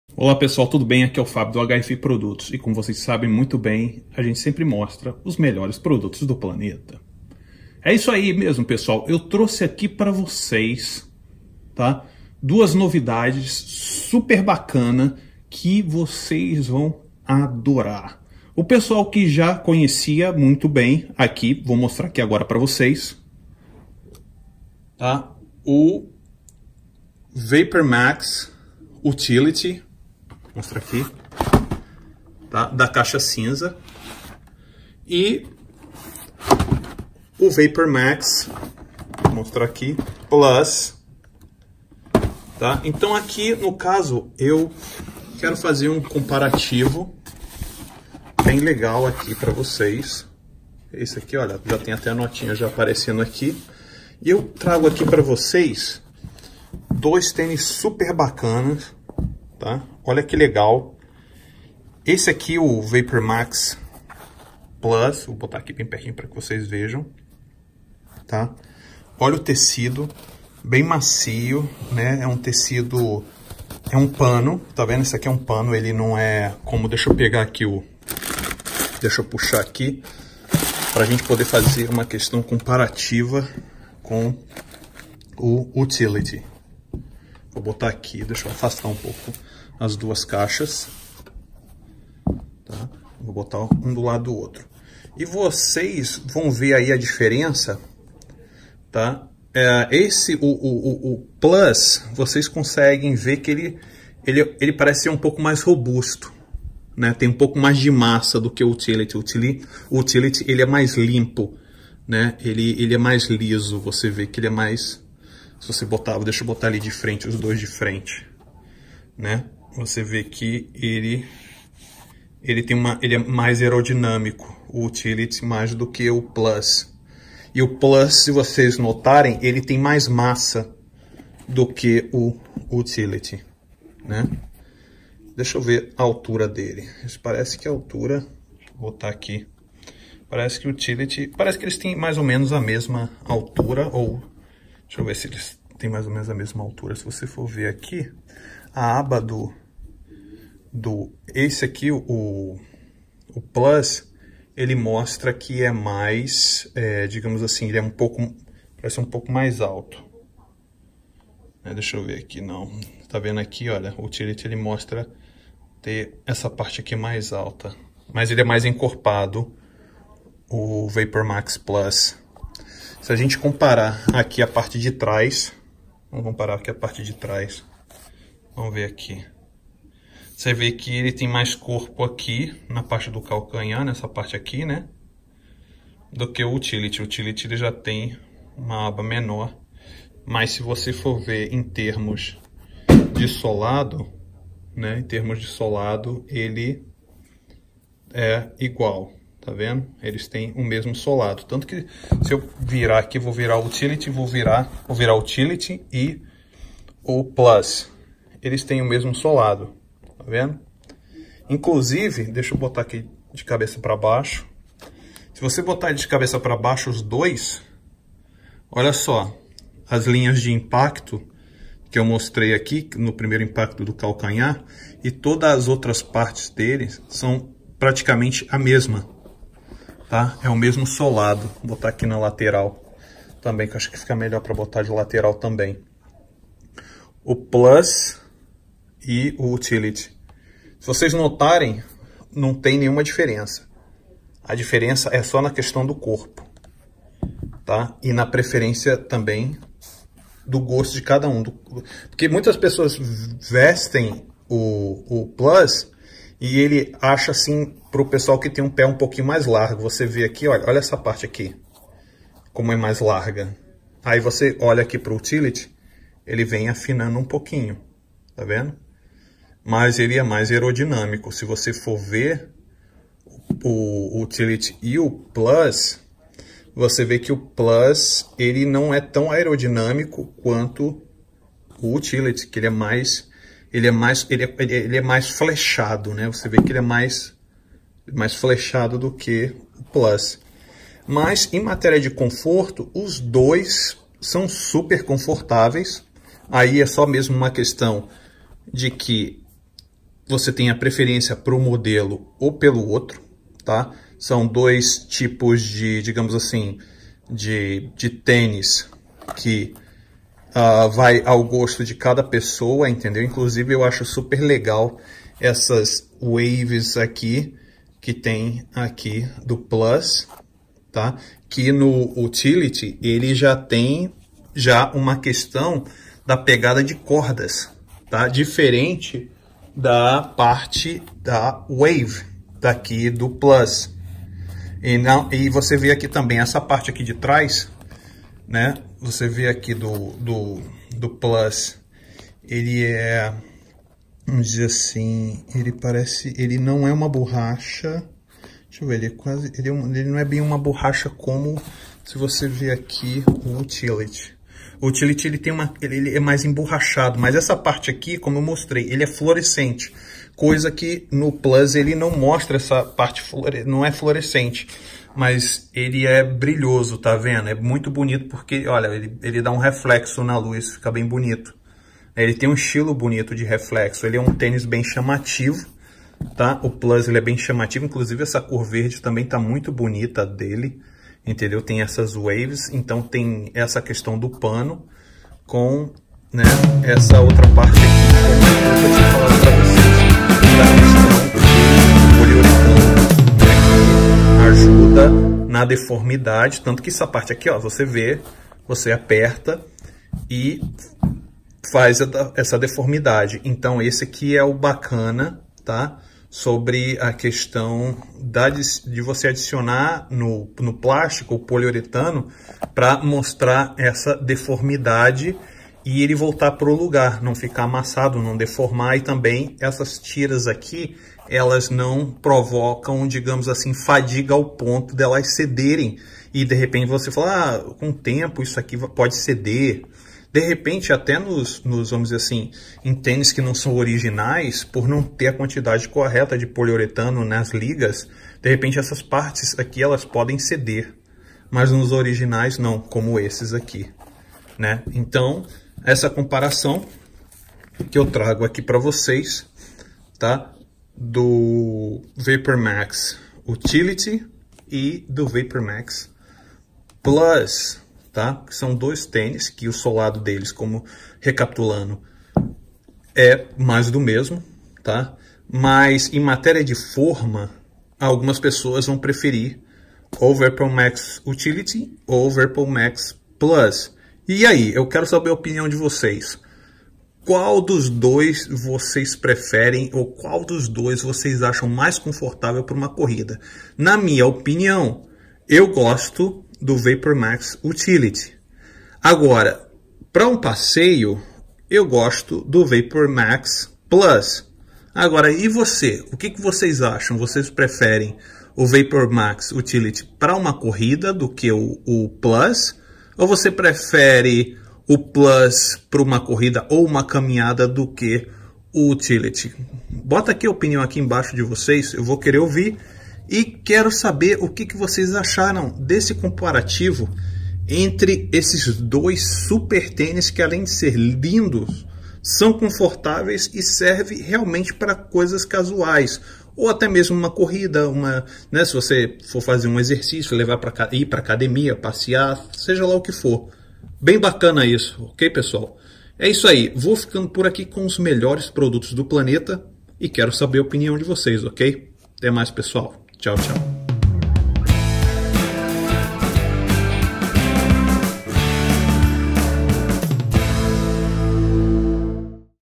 NIKE | Vapormax Run Utility VS Vapormax Plus.
Para o Pessoal fã da marca NIKE aqui está um super comparativo entre as duas versões do Vapormax então qual dos dois você acha mais poderoso?